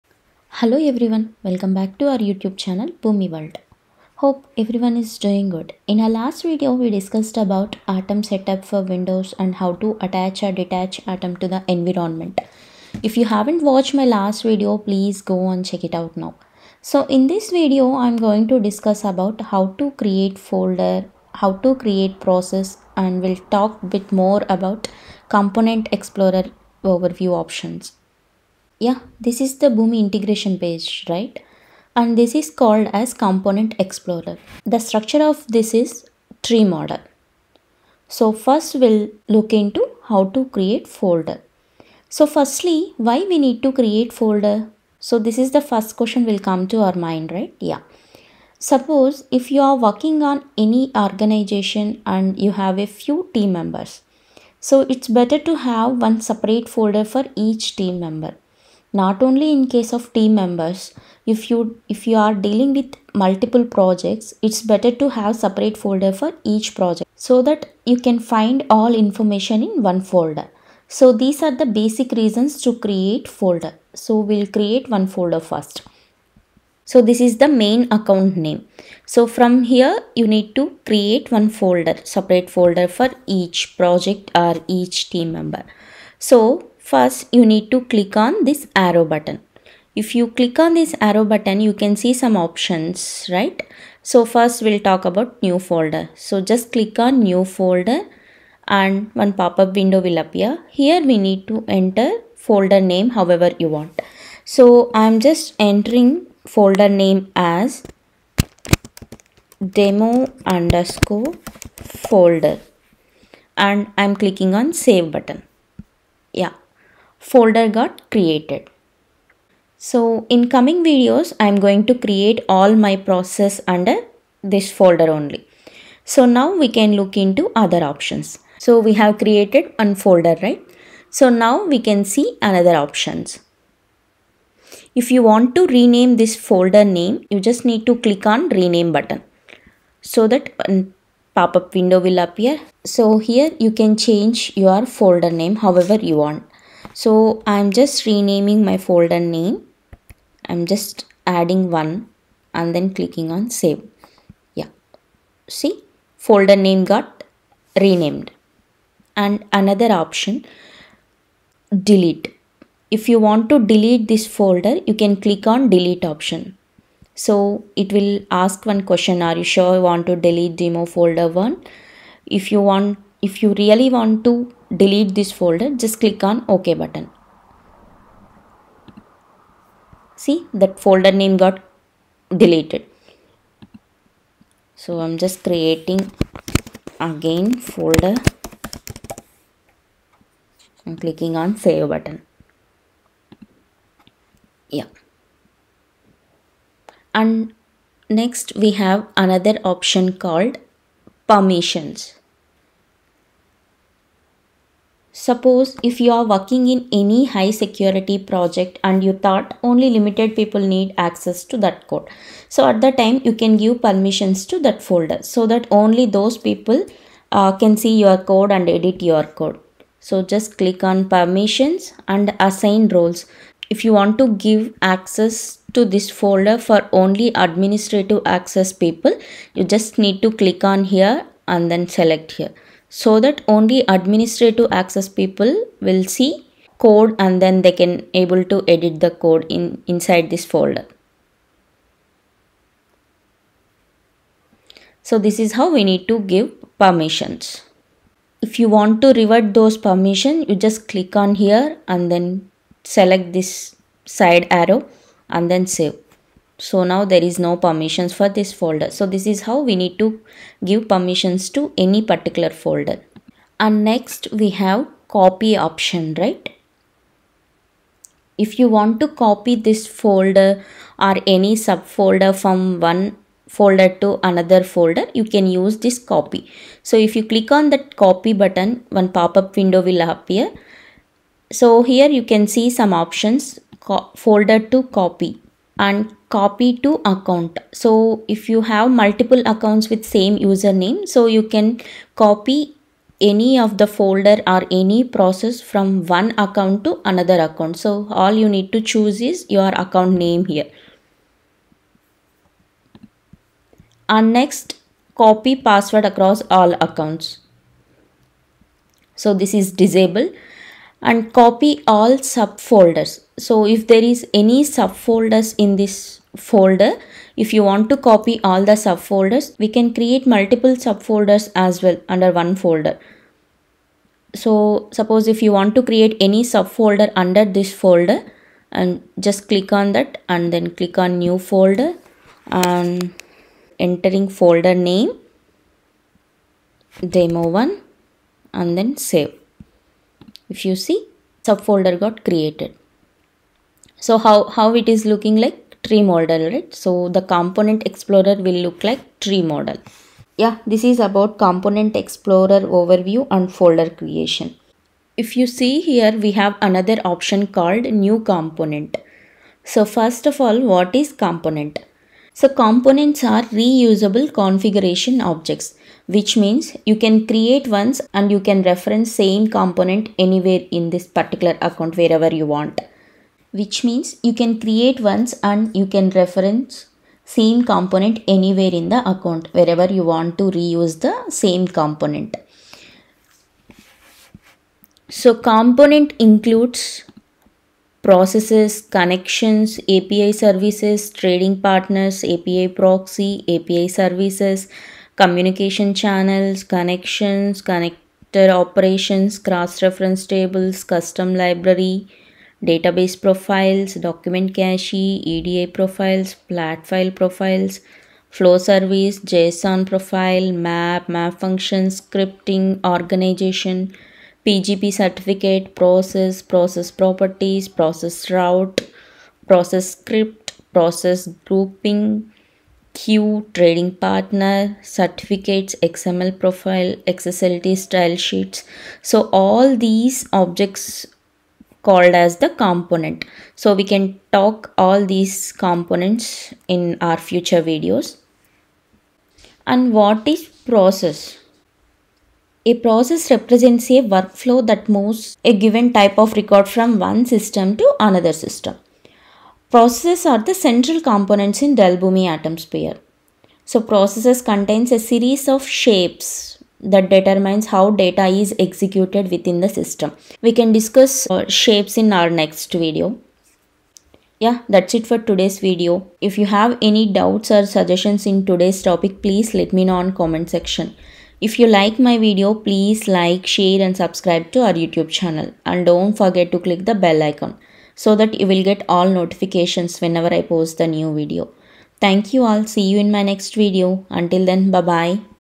Dell Boomi Tutorial Day-7 | Folder Creation | Process | Component | Boomi World
Hello Everyone,
In this video we are going to discuss about How to create folder, what is process and what is component and what are all the components in Dell Boomi.